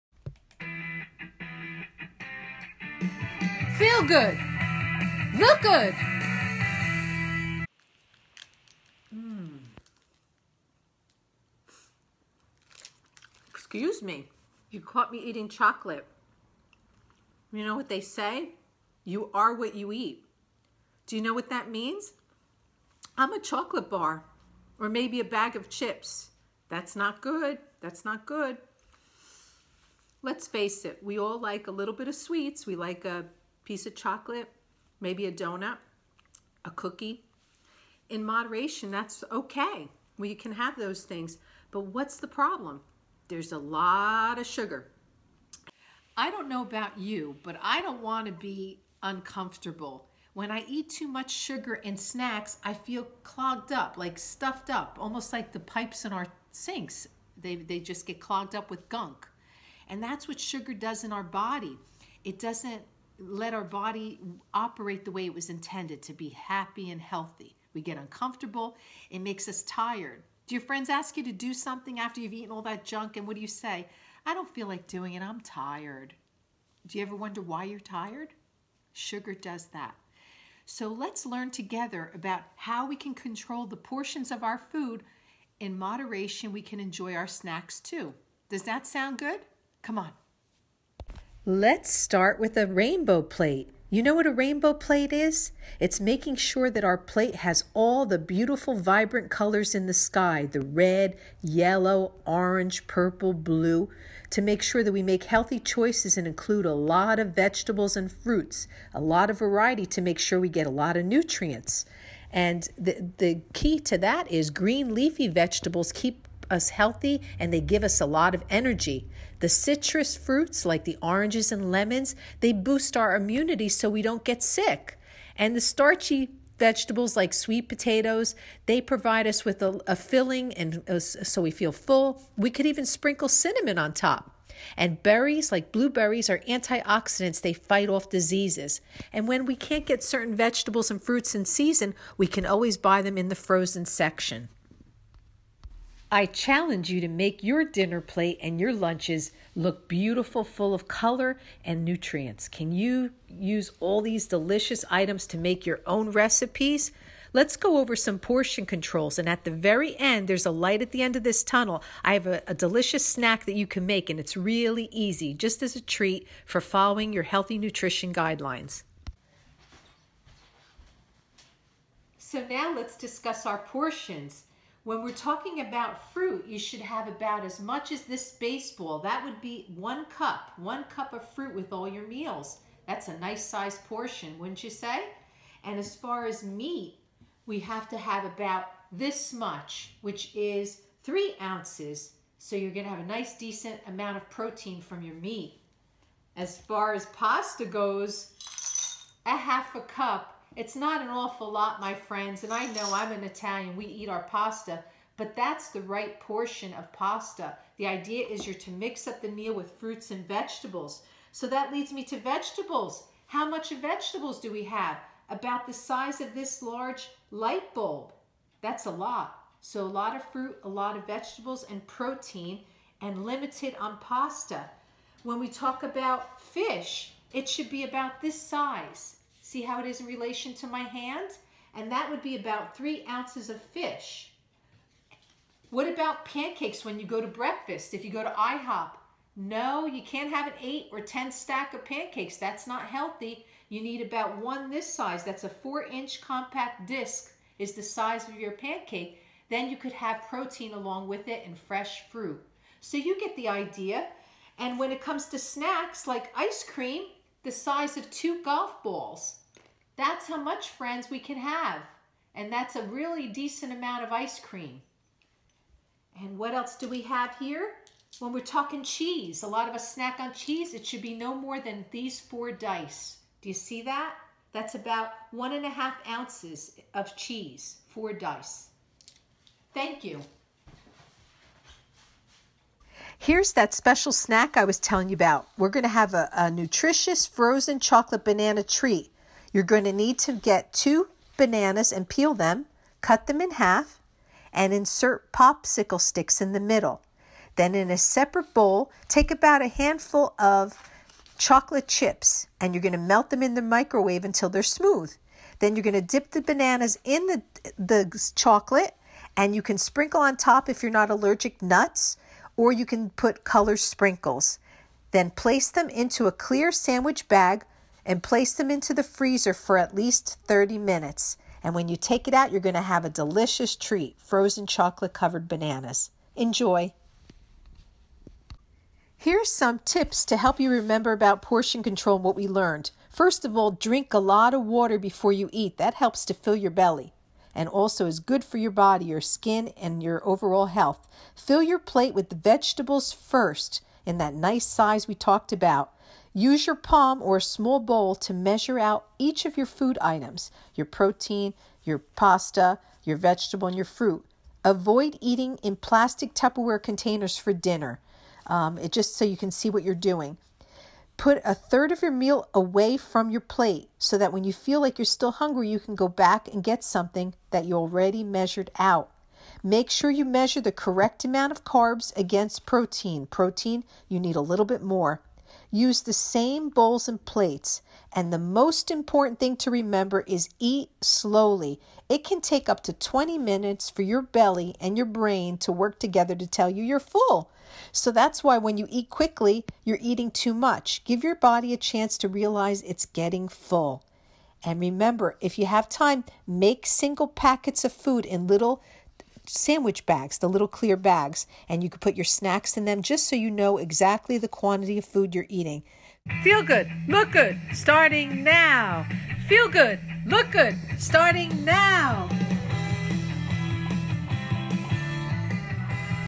Feel Good! Look Good! Food Portion Control
Joanne Bastante-Howard introduces adults with disabilities to nutritious eating and food portion control in a humorous way.